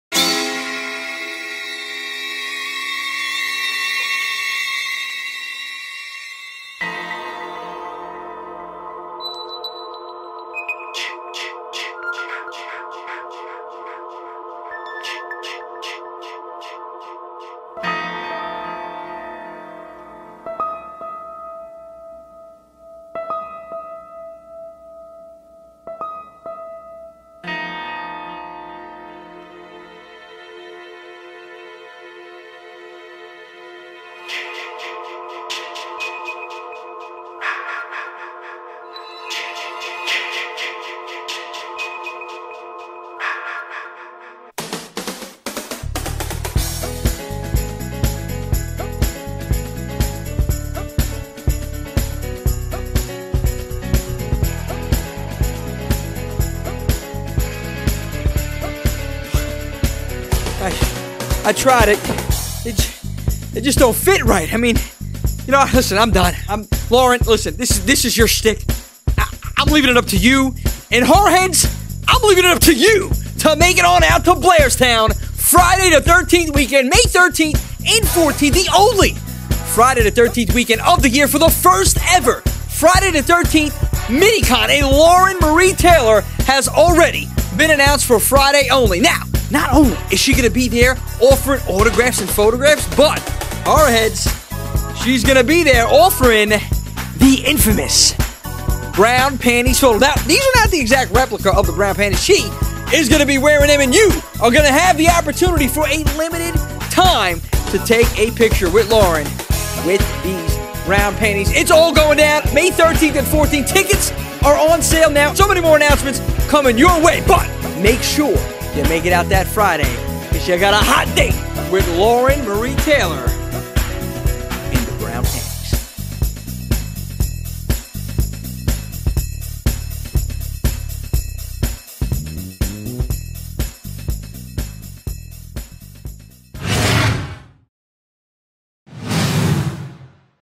Lauren Marie Taylor's SPECIAL Photo op!
Lauren Marie Taylor will be offering the Friday the 13th fans a special photo op at our FIRST EVER Friday the 13th Mini Con May 13th, 2022!

F13MINICON.EVENTBRITE.COM